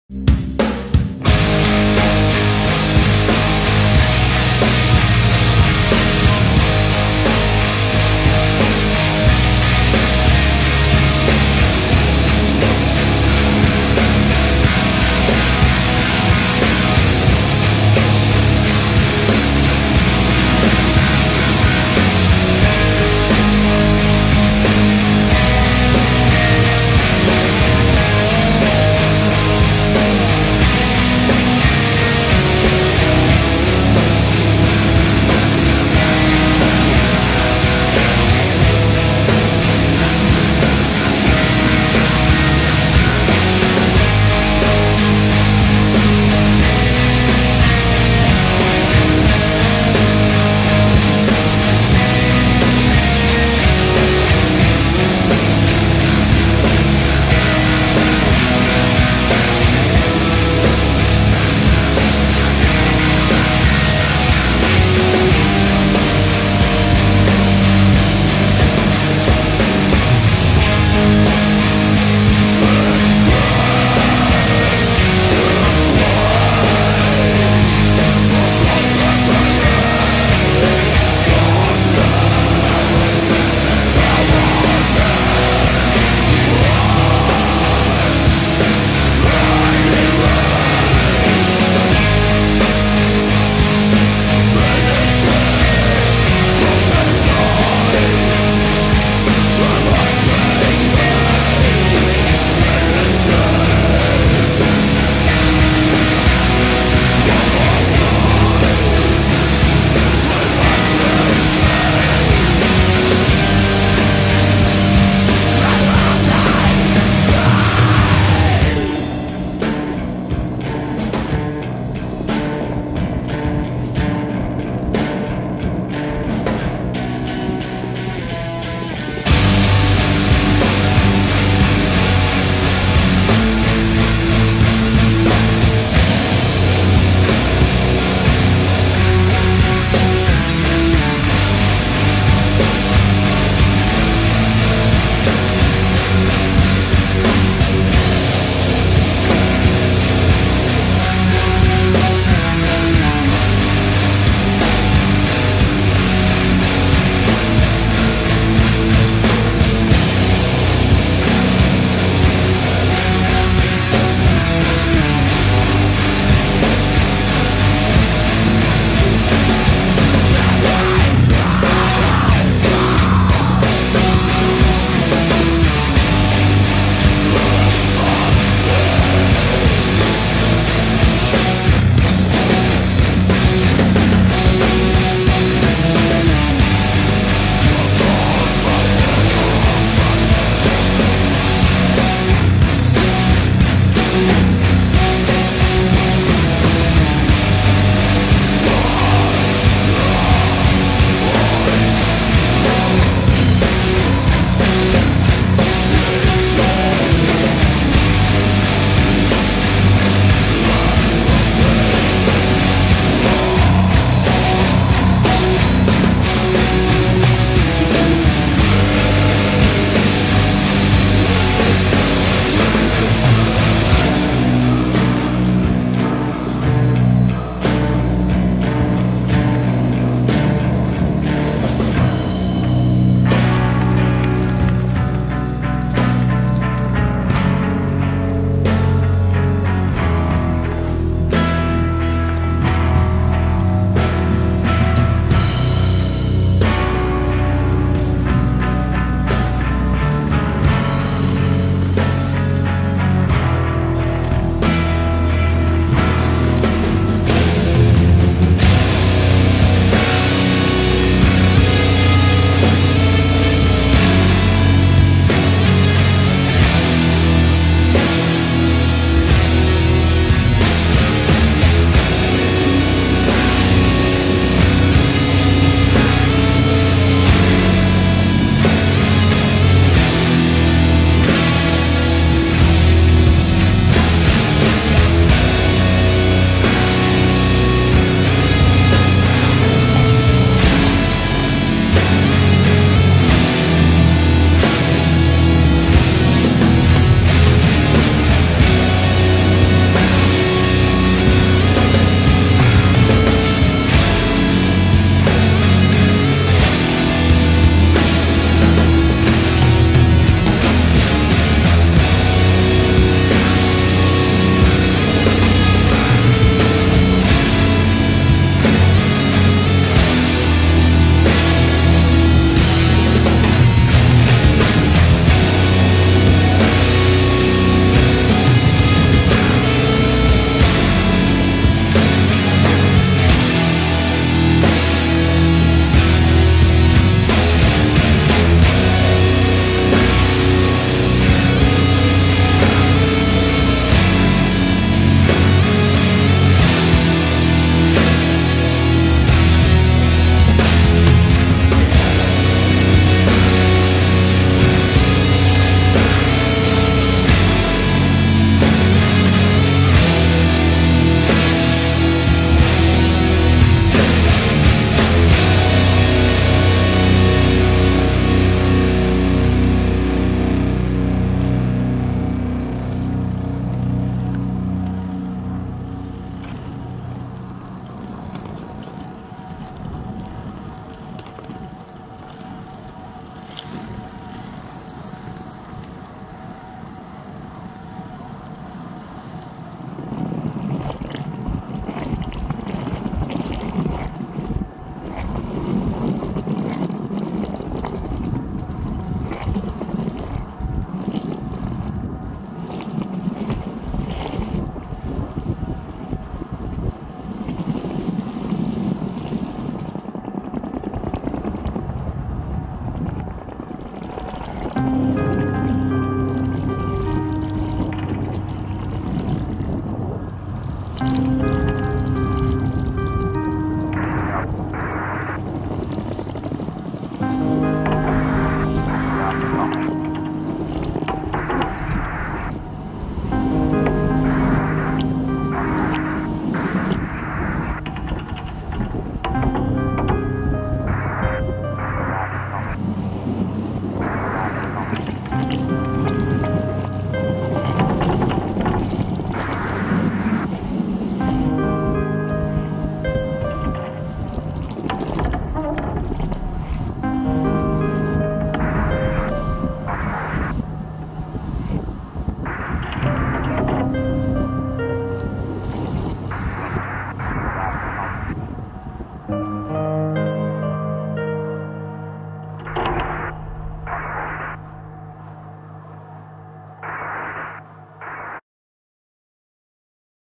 Pennywagens Cruisers #6 Kampen | Callisto
Pennywagens, onze zesde cruise met onze mini-cruisers van Ridge Skateboards, Kampen juli 2019. Wederom een zomeravond en wederom in Kampen, nu in het stadspark, langs de IJsselkade, over de stadsbrug (!) en door de Oude Straat. We rijden van licht naar donker en de gekozen muziek past daar fantastisch bij. Muziek: Callisto.